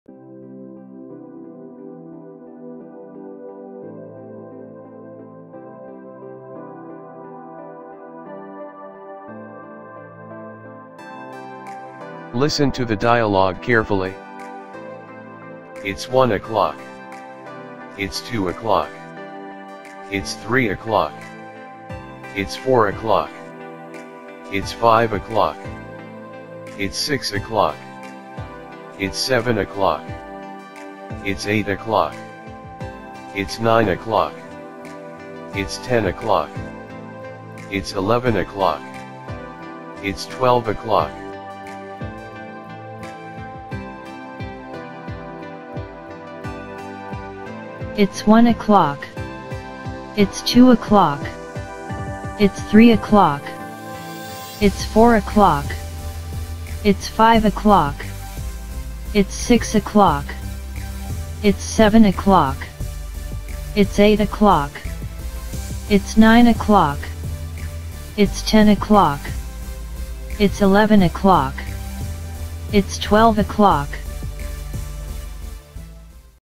Bahasa Inggris SMP When English Rings A Bell Kelas 7 Bab 3 Halaman 38
Media dibuat dari buku kurikulum 2013 When English Rings A Bell kelas 7 edisi revisi 2017 halaman 38